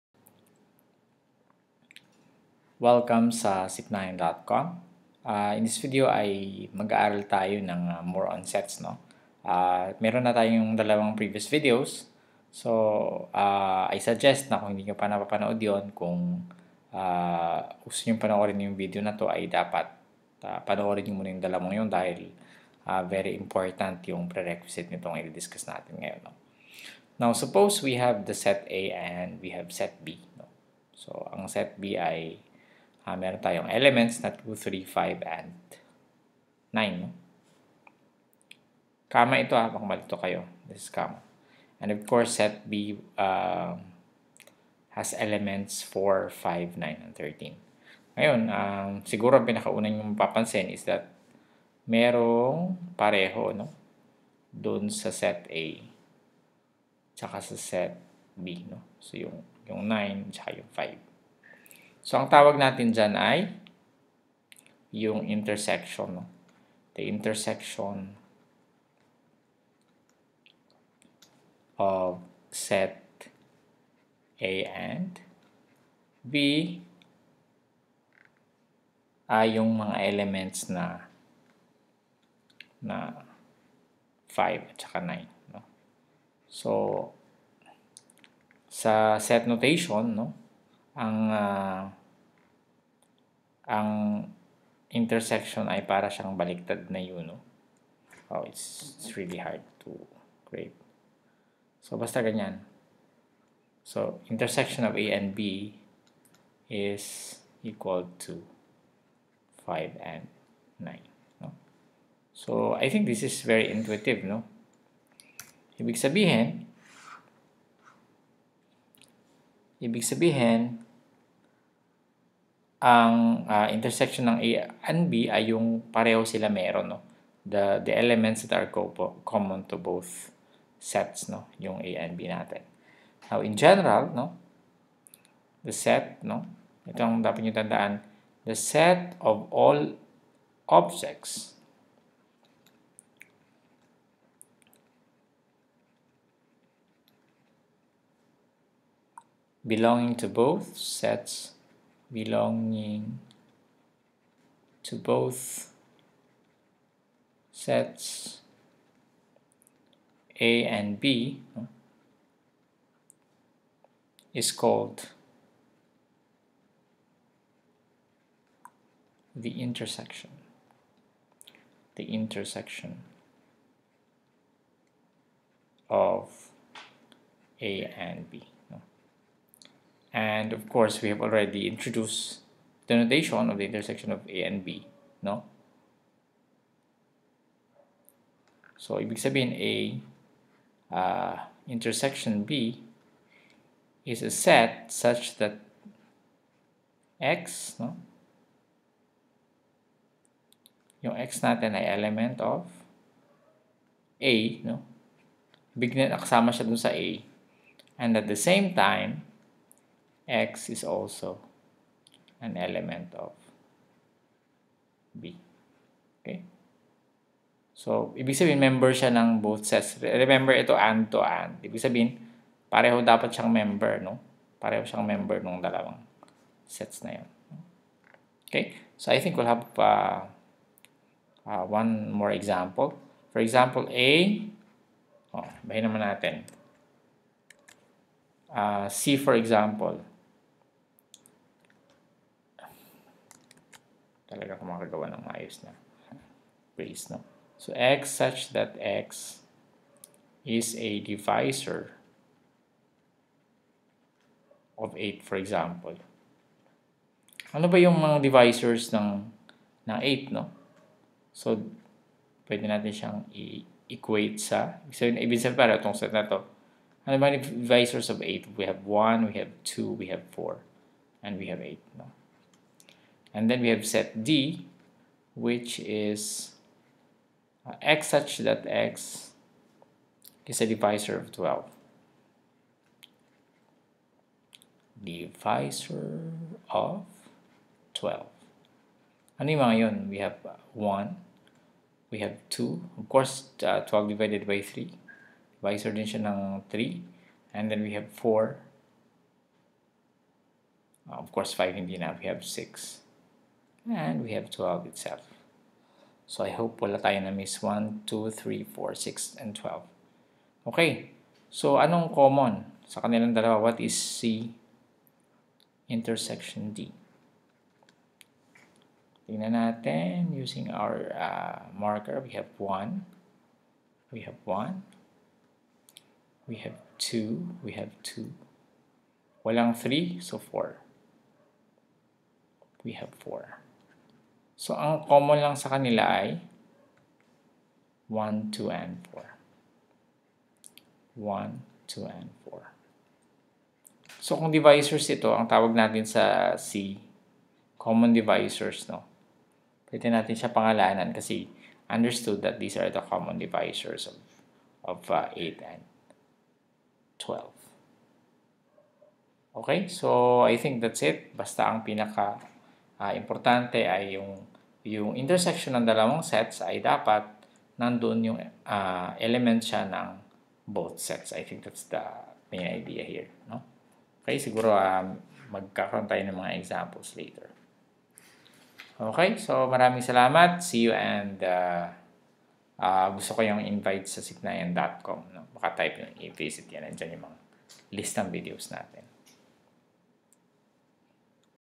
S03 Intersection of Sets Part 1
In this video, we discuss the intersection of sets. The set of all objects belong to both A and B is called the intersection of A and B.